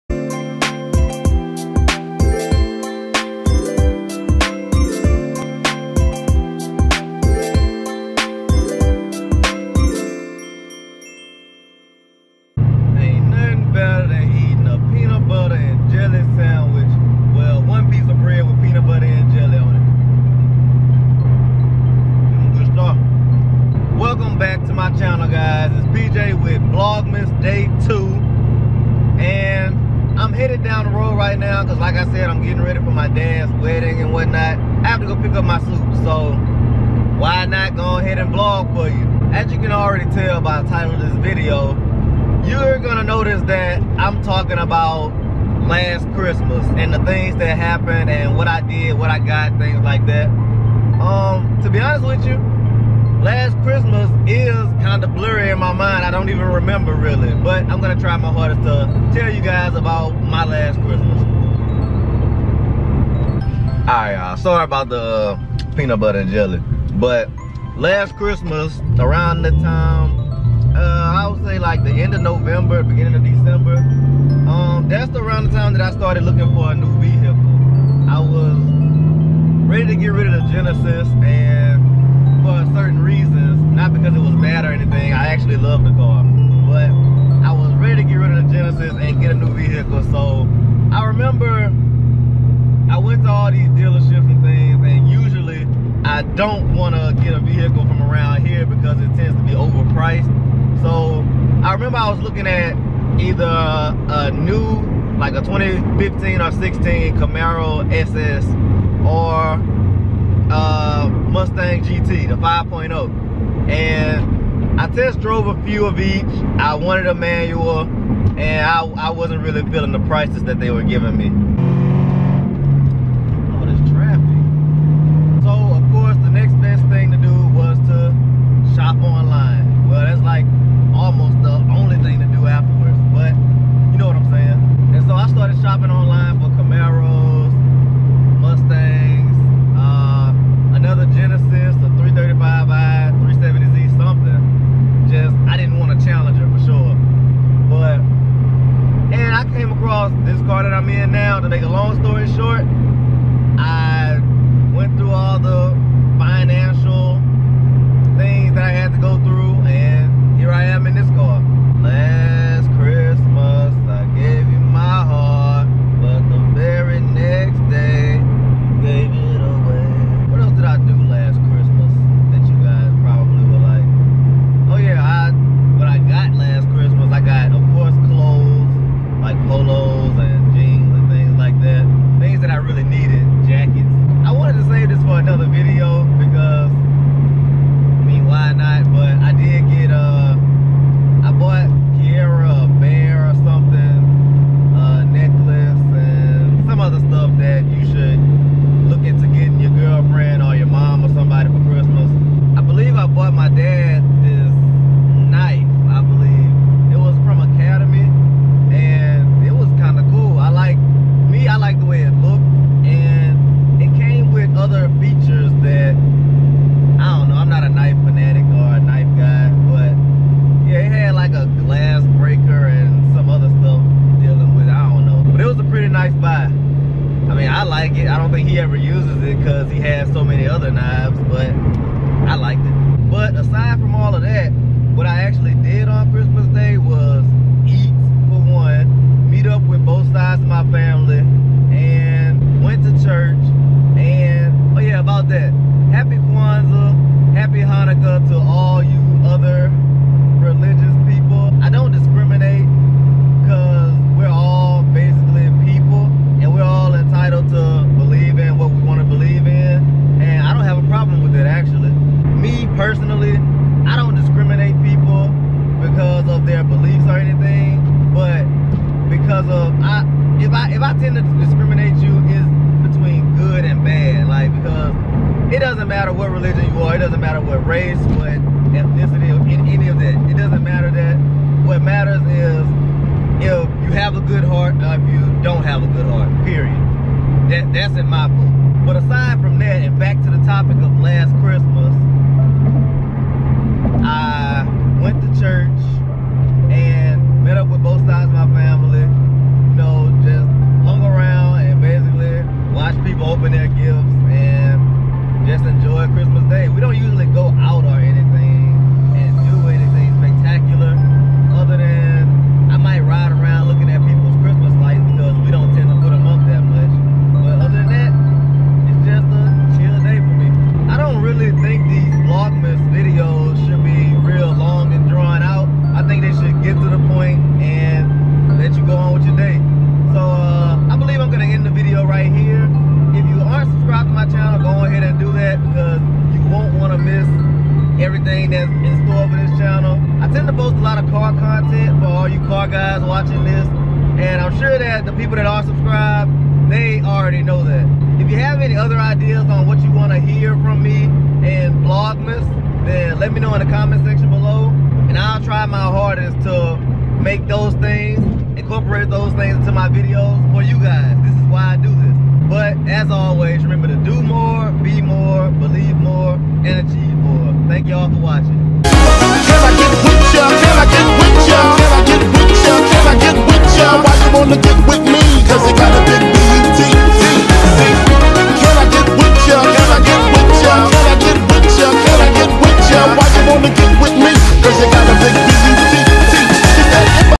Vlogmas Day 2 | About Last Christmas
Vlogmas Day 2 is here!

In this video, I explain some of the things that I did last Christmas and what made that one special. I hope you guys stick around to see what it is that I have to say about last Christmas!